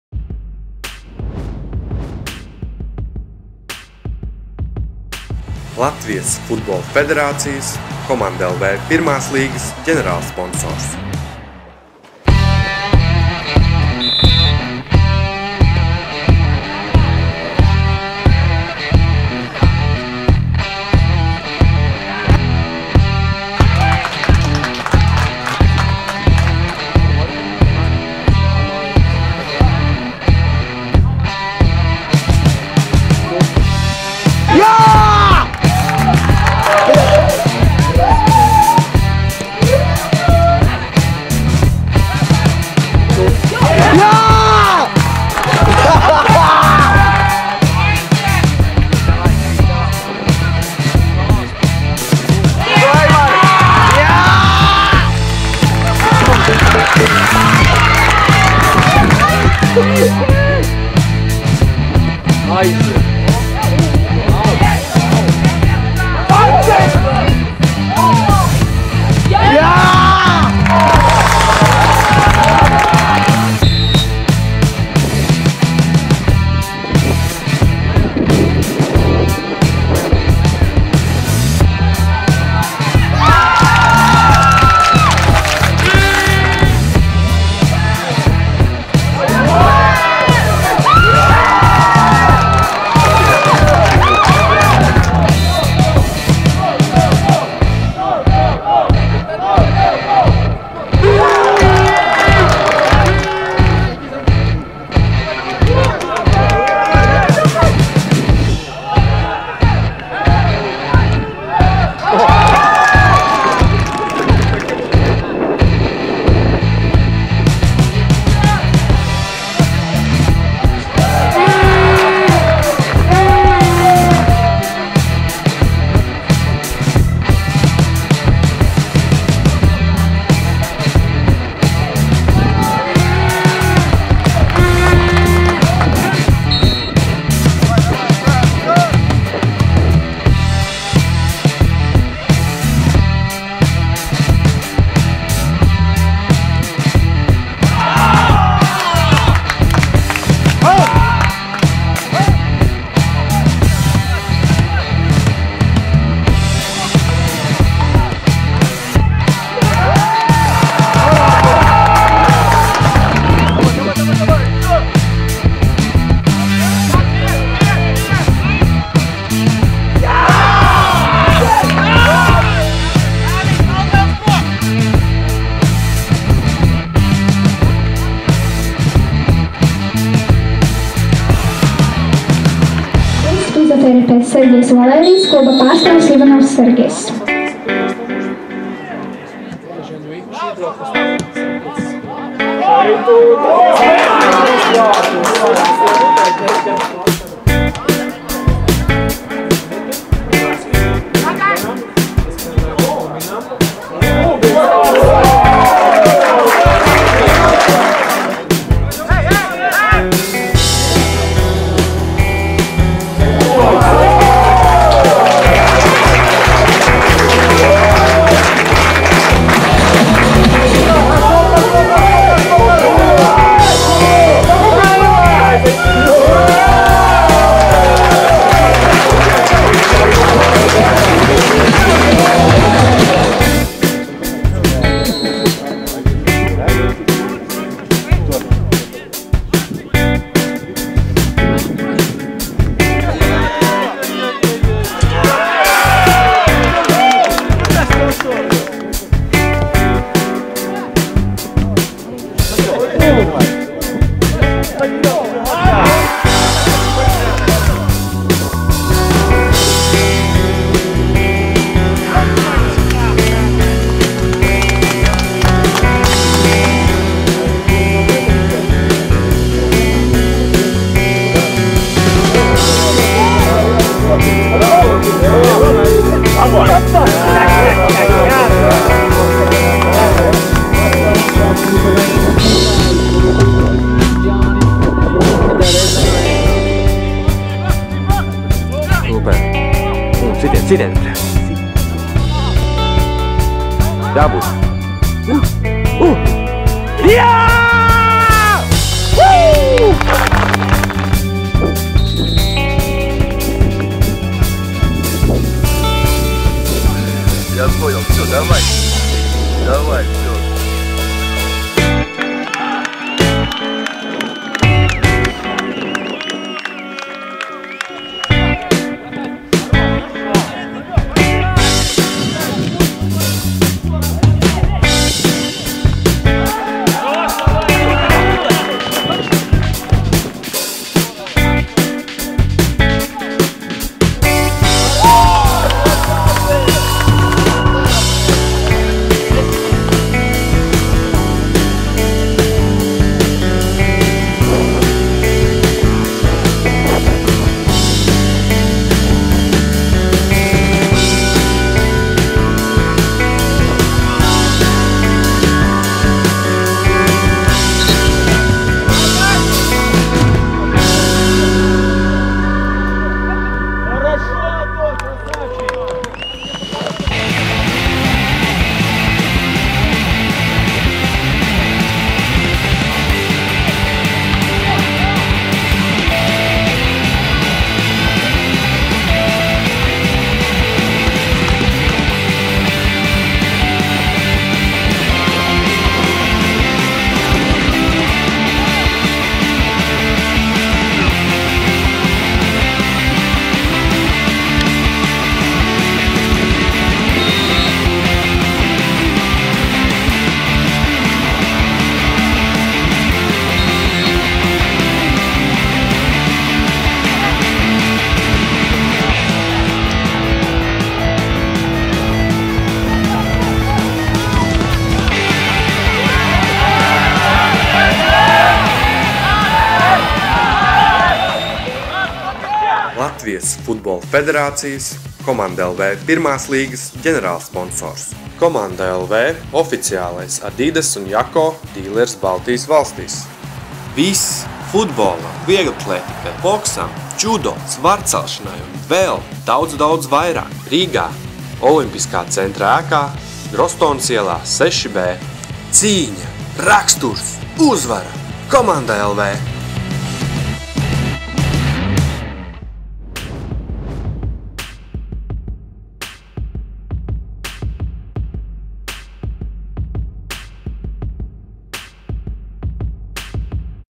komanda.lv 1. līgas čempionāts 2017 | 4. kārtas spēles
komanda.lv 1. līgas futbola čempionāta 2017. gada sezonas 4. kārtas spēļu apskats.